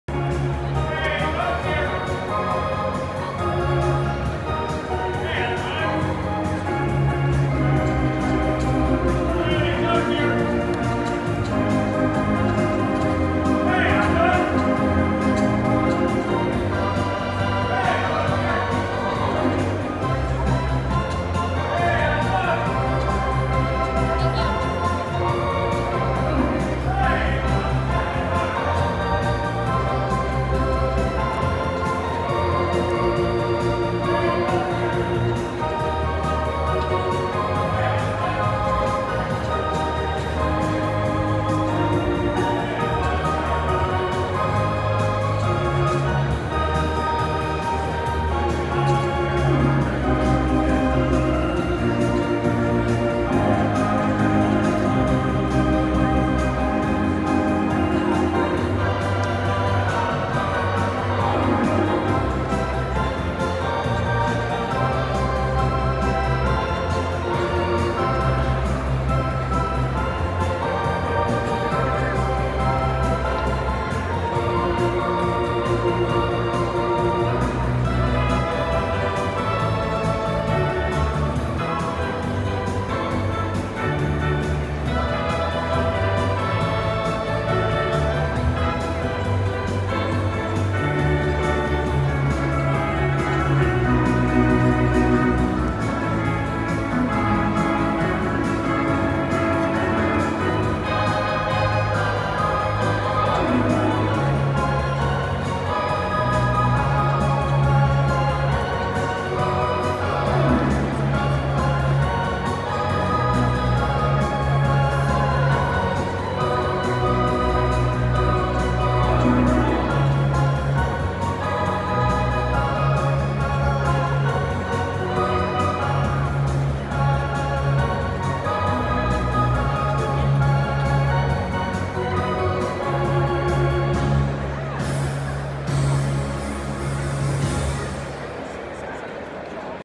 Sounds of Wrigley Field 2019. Organ music
Wrigley Field Organ playing before the game. Gary Pressy the organist.